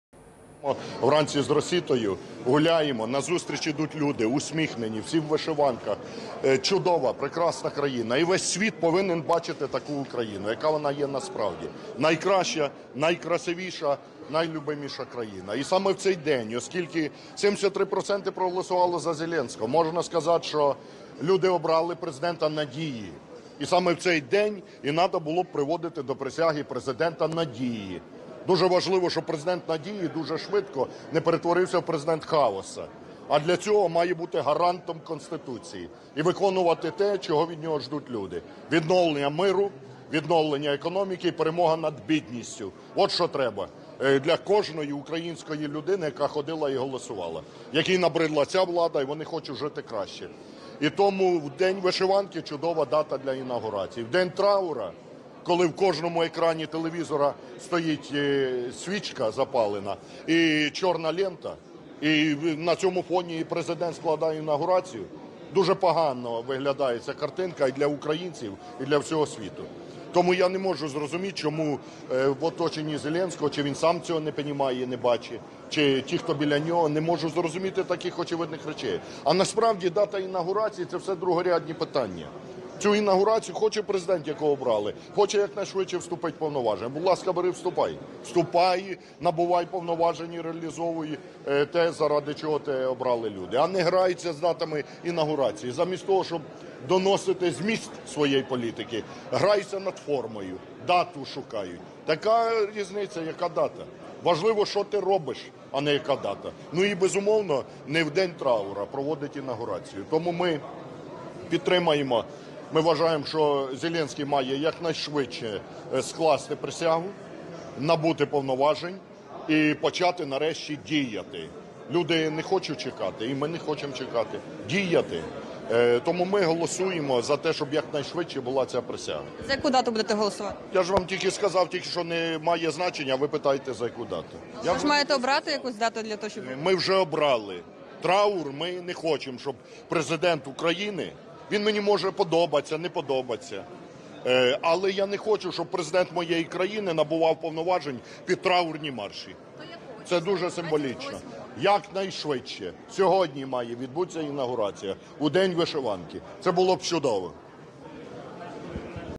Ляшко: Затягувати з інаугурацією не можна
Команда Олега Ляшка готова голосувати за будь-яку дату інавгурації новообраного Президента, окрім 19 травня – Дня пам’яті жертв політичних репресій. Про це розповів лідер Радикальної партії в кулуарах парламенту. Політик підкреслив, інавгурація – урочиста подія і має відбуватись у святковий день, а не у траурний.
«19 травня – День жалоби, коли в кожному екрані телевізора буде стояти запалена свічка і чорна стрічка. І на цьому фоні Президент складає присягу. Ця картинка погано виглядатиме і для українців, і для всього світу. Тому я не можу зрозуміти, чому в оточенні Зеленського не бачать таких очевидних речей», - сказав Ляшко.
Лідер РПЛ додав, що Україна – християнська держава і проведення інавгурації 19 числа, о 19 годині, 19 хвилині – неправильно, адже це кабалістична нумерологія.
«Дати і години інавгурації – це все другорядні питання. Хоче новообраний Президент якнайшвидше приступити до обов’язків - будь ласка, нехай набуває повноважень і реалізовує те, заради чого його обрали люди, а не грається з датами інавгурації. Люди не хочуть чекати. Треба діяти. А різниці, яка дата інавгурації, немає. Важливо те, що ти робиш», - підсумував Ляшко.